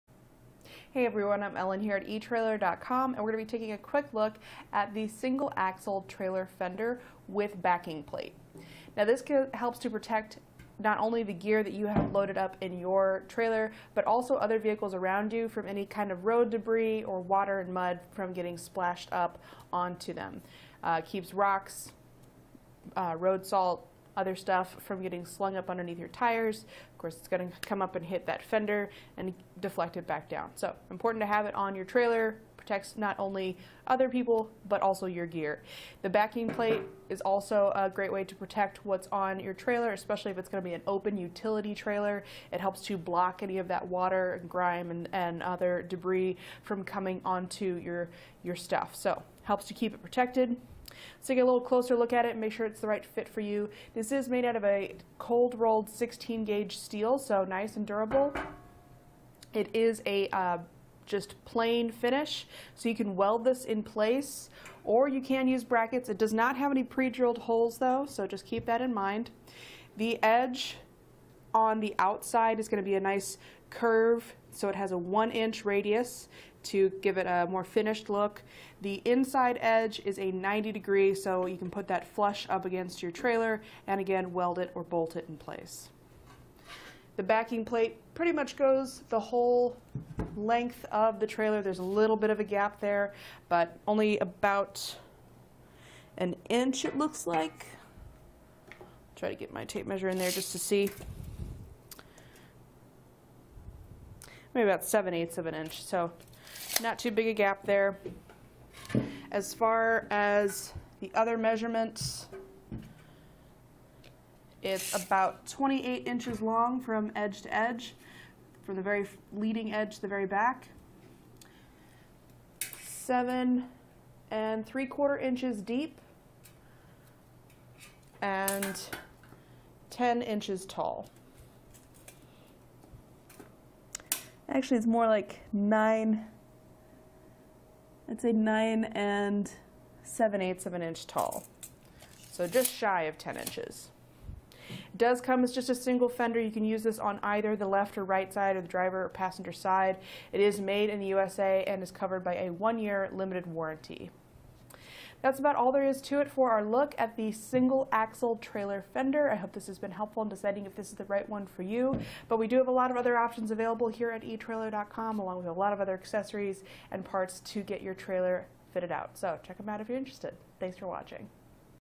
Review of HE Parmer Trailer Fenders - Steel Fender with Backing for 13-14 Inch Wheels - HP79VR
Ellen:                                                 Hey everyone. I'm Ellen here at etrailer.com and we're going to be taking a quick look at the single axle trailer fender with backing plate. Now this helps to protect not only the gear that you have loaded up in your trailer, but also other vehicles around you from any kind of road debris or water and mud from getting splashed up onto them, keeps rocks, road salt, other stuff from getting slung up underneath your tires. Of course, it's going to come up and hit that fender and deflect it back down. So important to have it on your trailer, protects not only other people, but also your gear. The backing plate is also a great way to protect what's on your trailer.

Especially if it's going to be an open utility trailer, it helps to block any of that water and grime and other debris from coming onto your stuff. So helps to keep it protected.So get a little closer look at it and make sure it's the right fit for you. This is made out of a cold rolled, 16 gauge steel, so nice and durable. It is a just plain finish. You can weld this in place or you can use brackets.It does not have any pre drilled holes though, just keep that in mind.

The edge on the outside is going to be a nice curve. It has a one inch radius to give it a more finished look. The inside edge is a 90 degree, so you can put that flush up against your trailer and again, weld it or bolt it in place.The backing plate pretty much goes the whole length of the trailer. There's a little bit of a gap there, but only about an inch it looks like. Try to get my tape measure in there just to see.

Maybe about seven eighths of an inch. Not too big a gap there. As far as the other measurements, it's about 28 inches long from edge to edge from the very leading edge, the very back. Seven and three quarter inches deep and 10 inches tall.Actually it's more like nine, I'd say nine and seven eighths of an inch tall. So just shy of 10 inches.

It does come with just a single fender. You can use this on either the left or right side of the driver or passenger side. It is made in the USA and is covered by a one-year limited warranty.That's about all there is to it for our look at the single axle trailer fender. I hope this has been helpful in deciding if this is the right one for you, but we do have a lot of other options available here at etrailer.com, along with a lot of other accessories and parts to get your trailer fitted out. So check them out if you're interested.